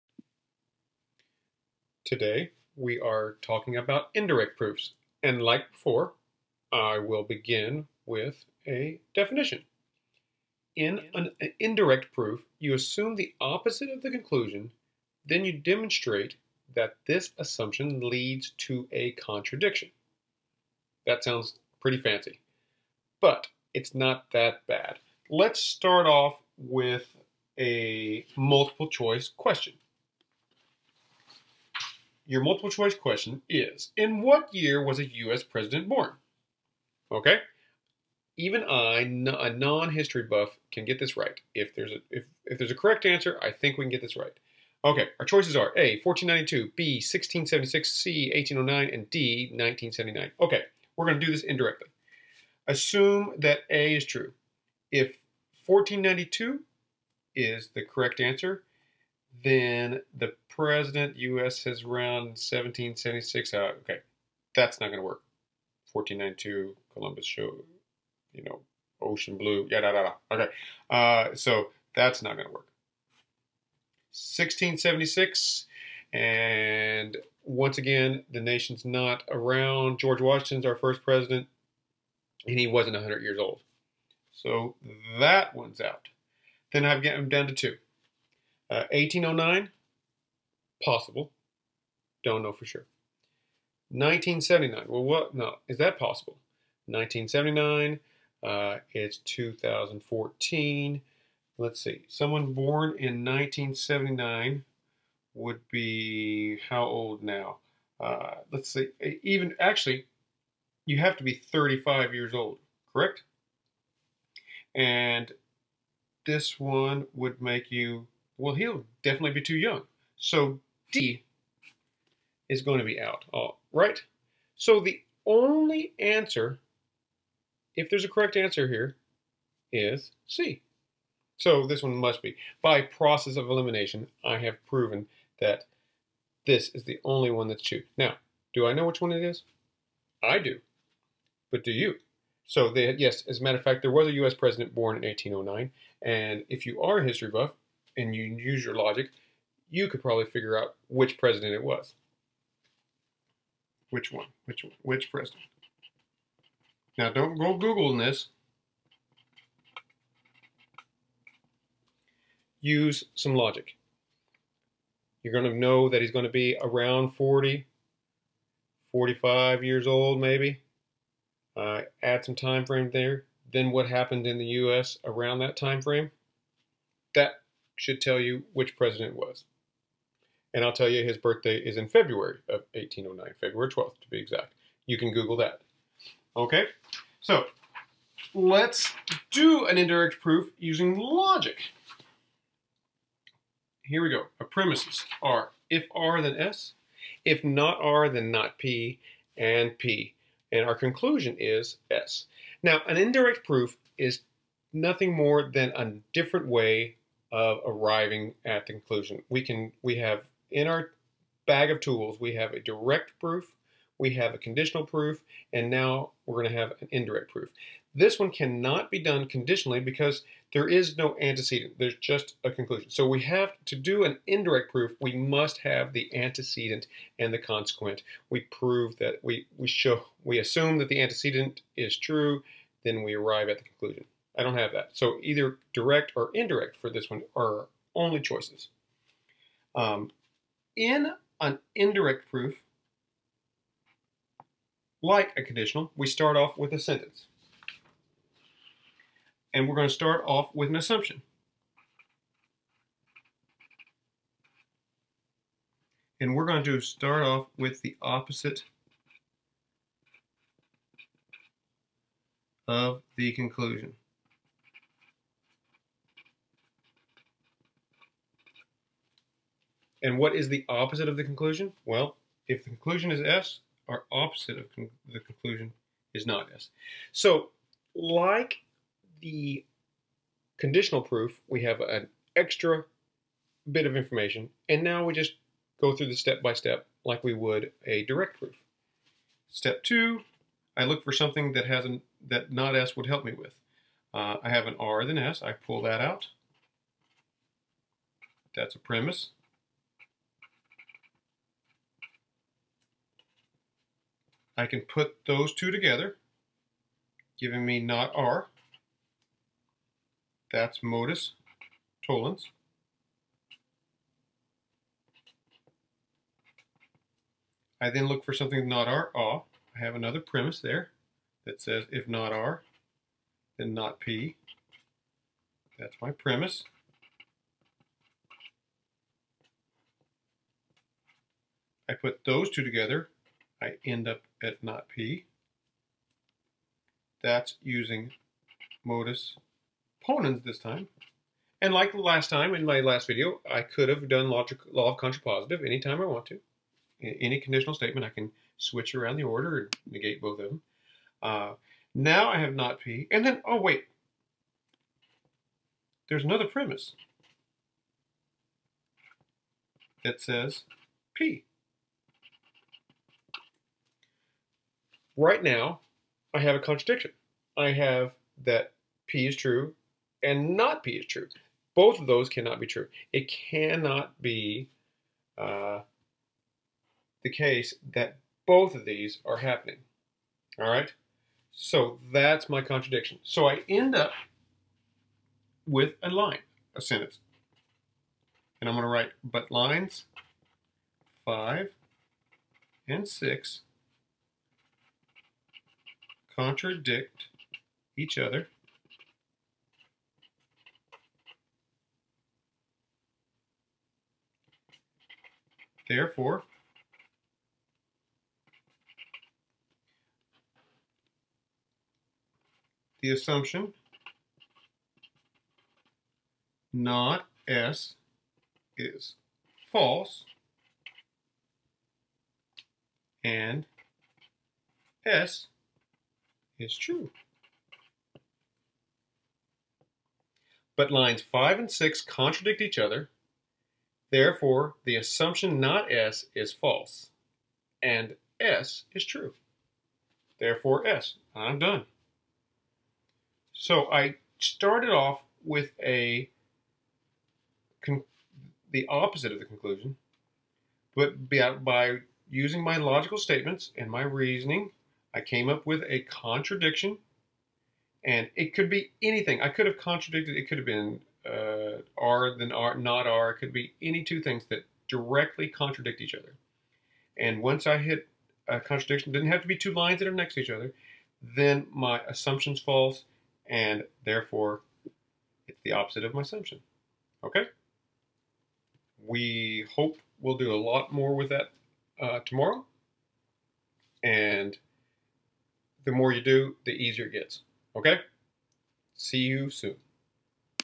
Indirect Proof
The third type of logic proof is an indirect proof